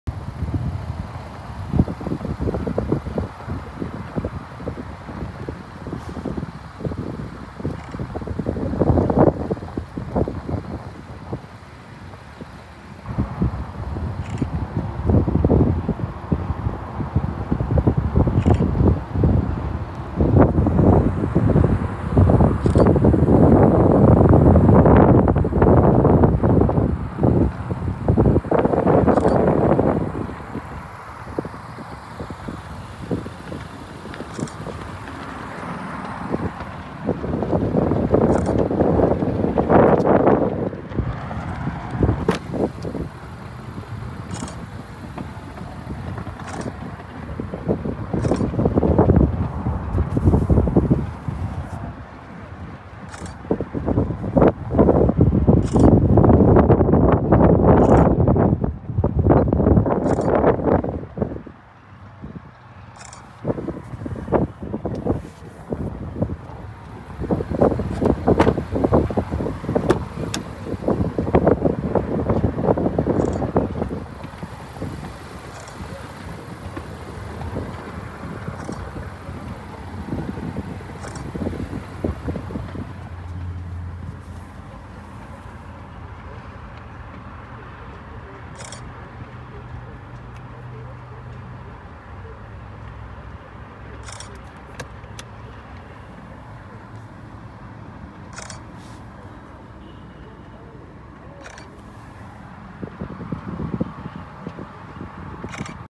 2021 Nissan Rogue Sport Weatherford, Fort Worth, Granbury, Saginaw, Dallas, TX MW300474
Gun Metallic New 2021 Nissan Rogue Sport available in Fort Worth, Texas at Fort Worth Nissan. Servicing the Weatherford, Granbury, Saginaw, Dallas, TX area.
2021 Nissan Rogue Sport SL - Stock#: MW300474 - VIN#: JN1BJ1CV8MW300474

3451 West Loop 820 S

Gun Metallic 2021 Nissan Rogue Sport SL FWD CVT with Xtronic 2.0L DOHCbrbrRecent Arrival! 25/32 City/Highway MPG